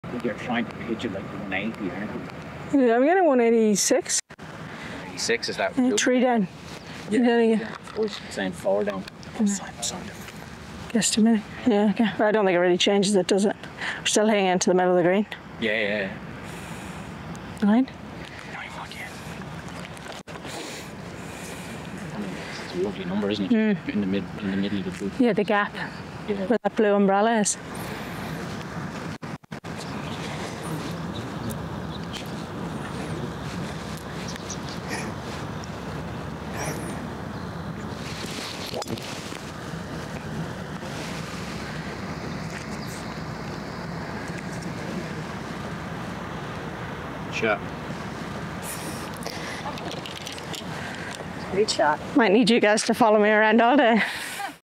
Leona Maguire Plays Pebble Beach's 8th Hole
Danger: Steep Cliff. ProV1 player Leona Maguire made Pebble Beach’s 8th hole look easy (the first time she ever played it!) when we joined her inside the ropes during her Monday practice round at the U.S. Women's Open.